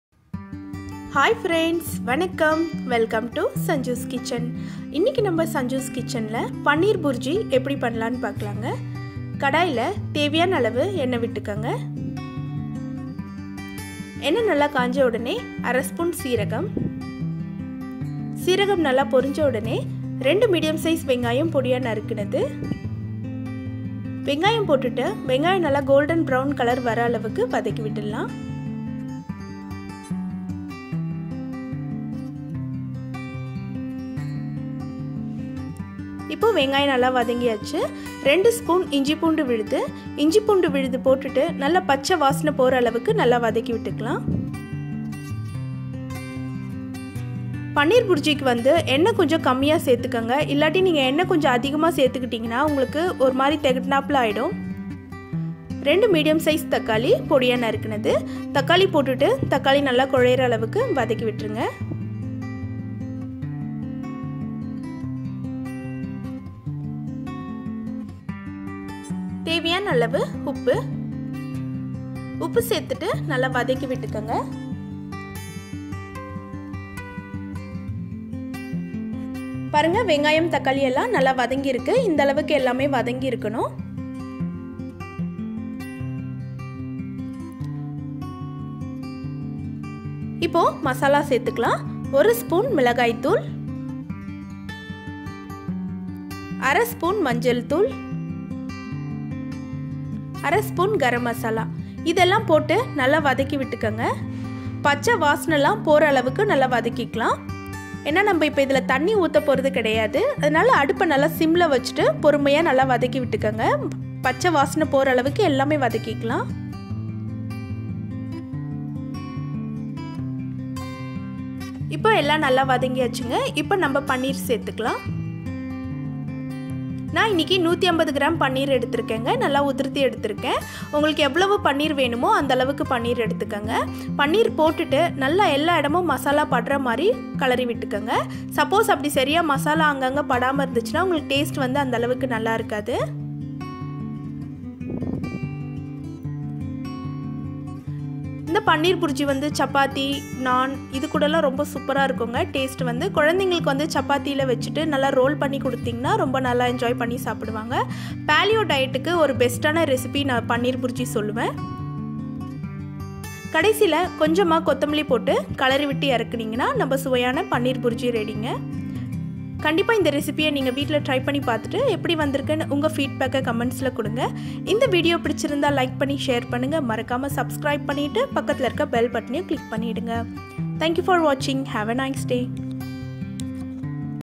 Paleo Diet Paneer Bhurji / Paneer Bhurji in Tamil / பன்னீர் புர்ஜி /
This video how to make Paneer Bhurji in tamil. This recipe easy, tasty & Healthy. Paneer Bhurji is best recipe for paleo diet.